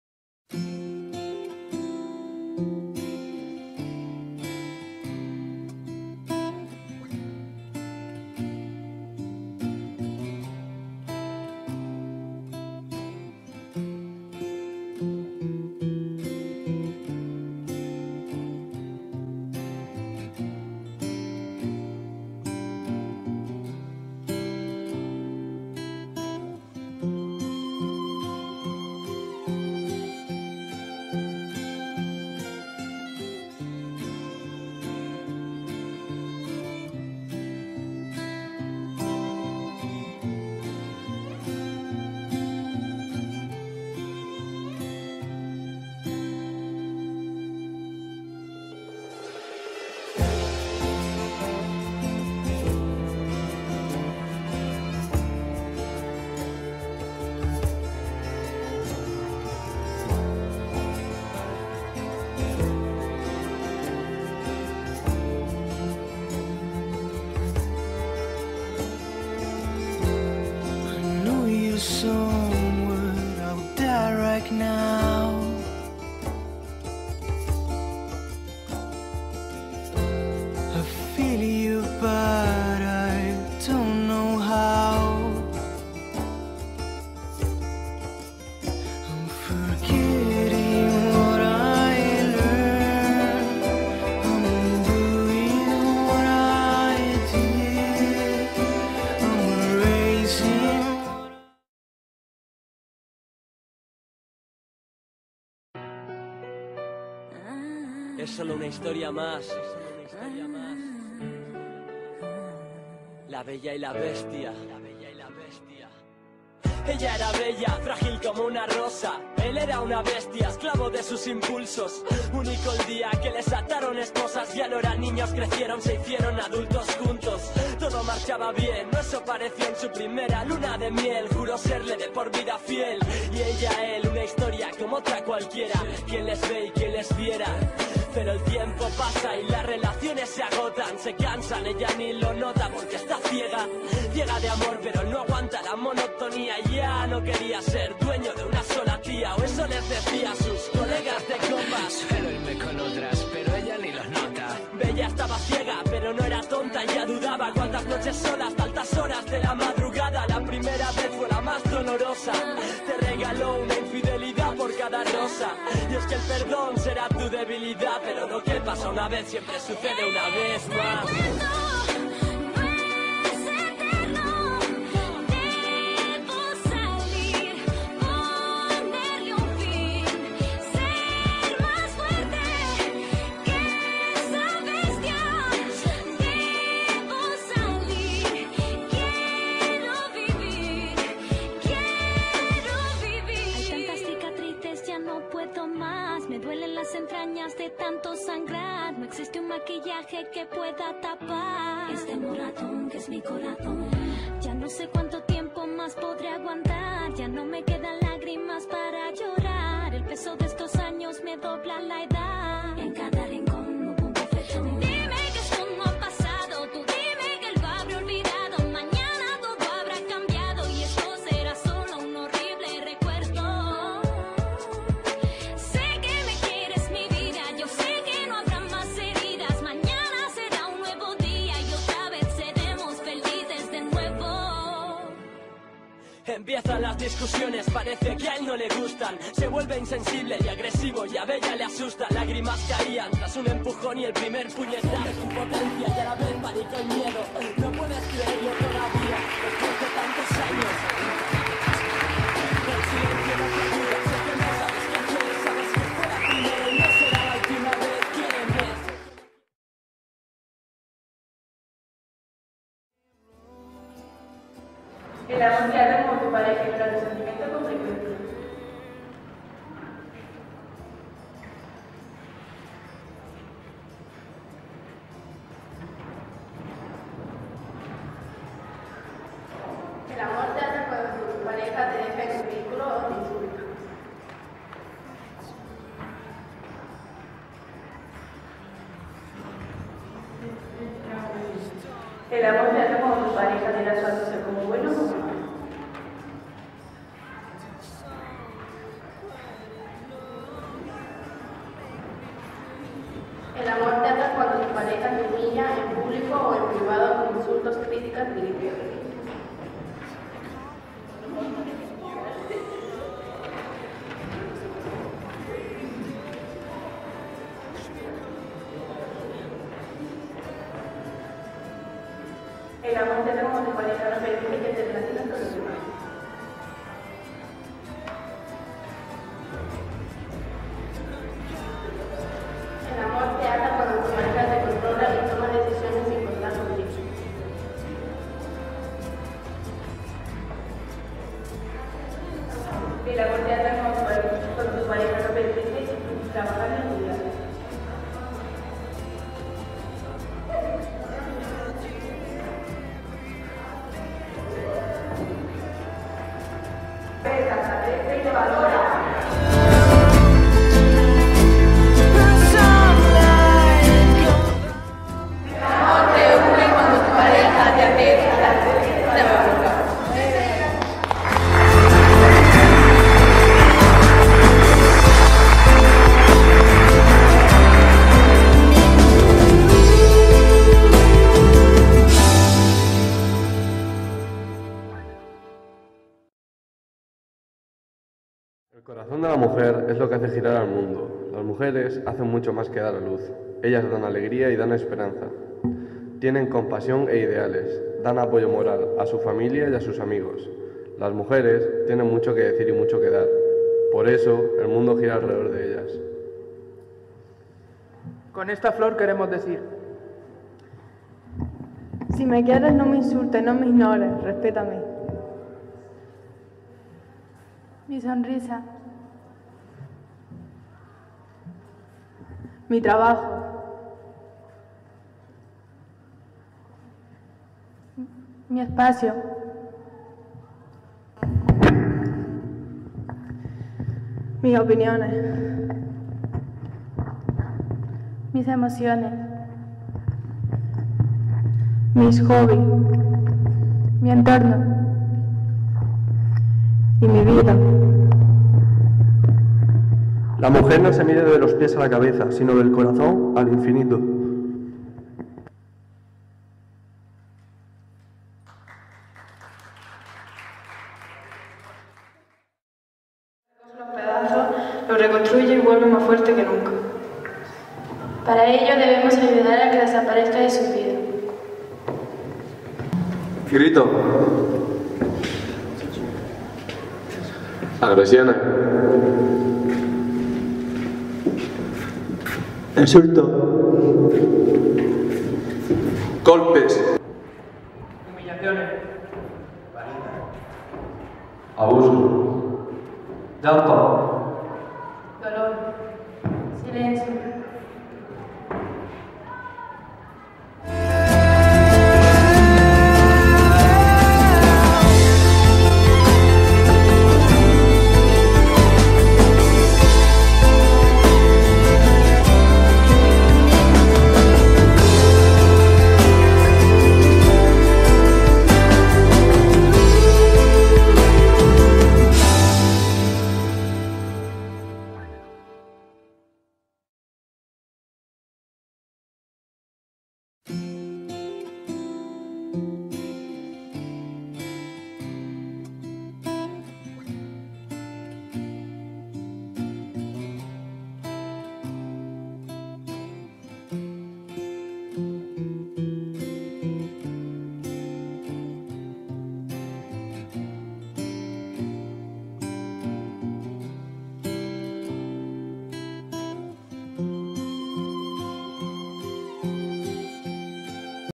Actividades para la Igualdad y contra la violencia Machista en Mazarrón
Vídeo elaborado con los alumnos y alumnas del IES Felipe II y usuarios/as del Centro de Día de Personas con Discapacidad de Mazarrón (Murcia) llevaron a cabo con motivo del Día Internacional contra la Violencia de género una serie de actuaciones musicales y teatrales para reivindicar relaciones de pareja desde la igualdad de trato. La iniciativa se ha organizado con el CAVI y el Ayuntamiento de Mazarrón y la colaboración del área de Igualdad de la Fundación Cepaim.